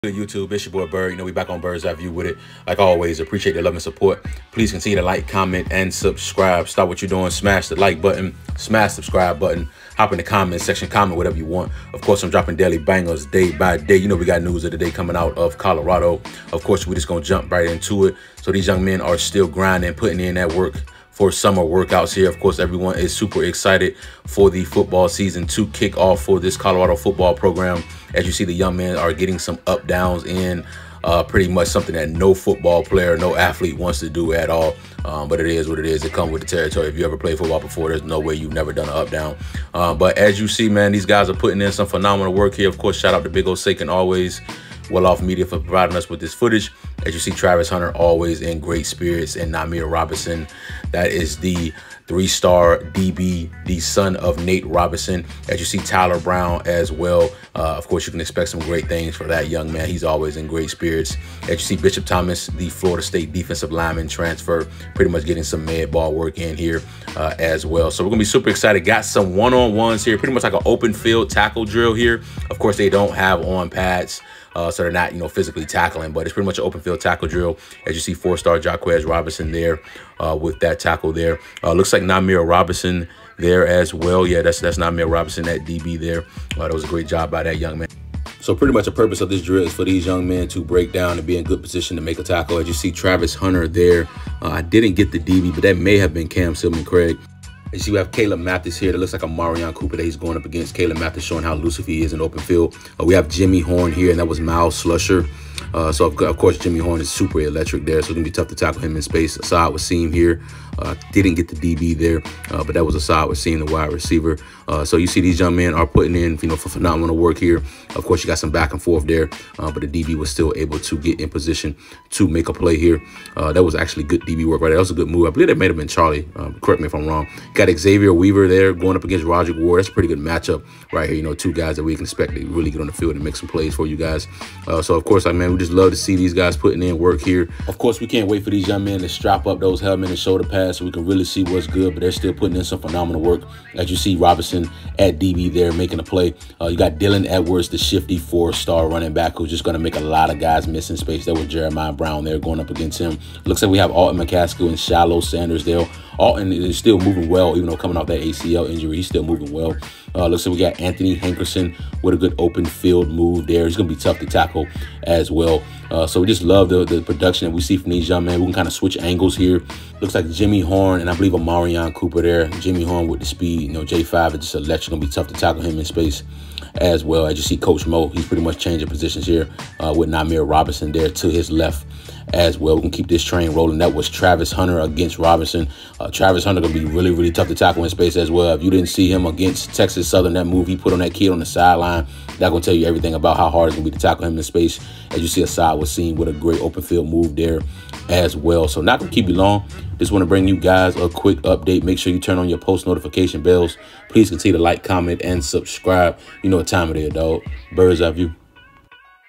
1v1s OPEN FIELD TACKLE DRILL!! COLORADO FOOTBALL LOUIS LUGGAGE SUMMER WORKOUT!! COACH PRIME!!
Colorado Football with a 1v1 open field tackle drill today during summer workouts!!

S/O: @welloffmedia ‼️‼️